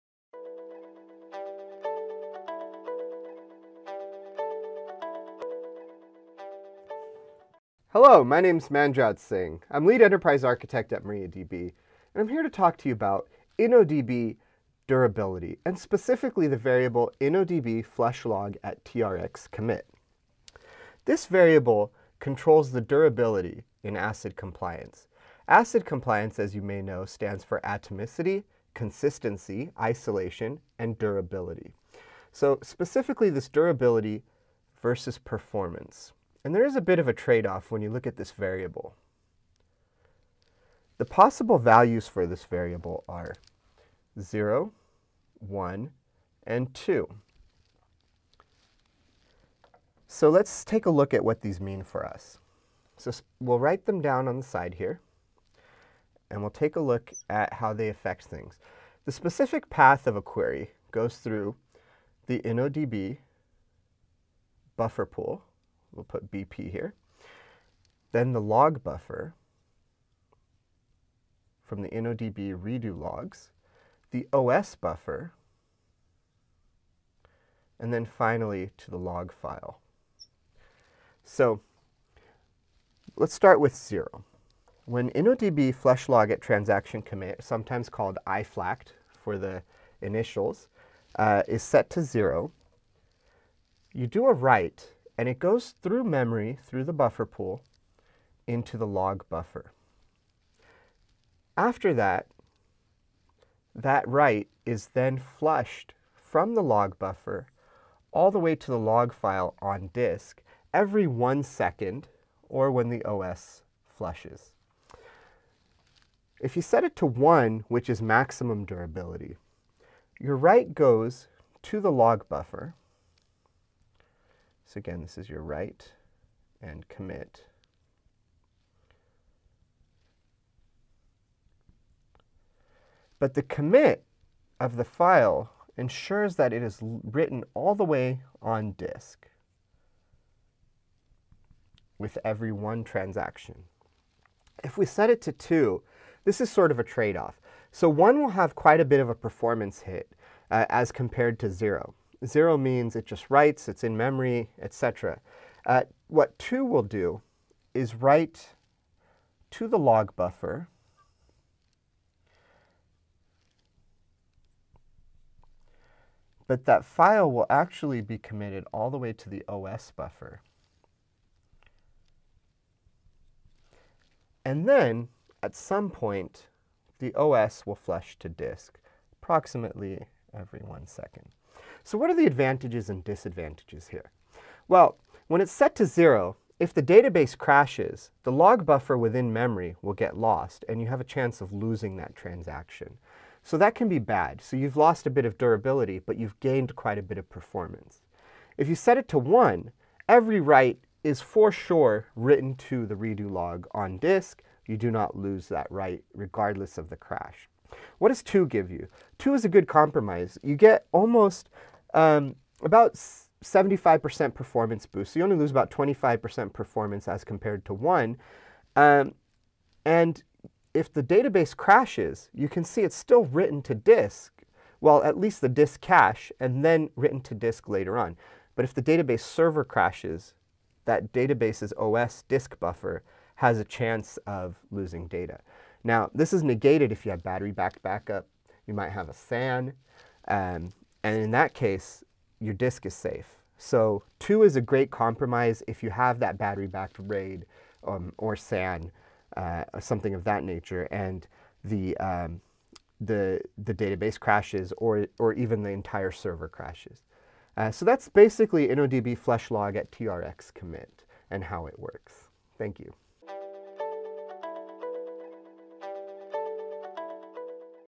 InnoDB Commits
Learn about how InnoDB Flush Log At Trx Commit works in MariaDB. It's an complex, interesting variable that can effect durability and performance.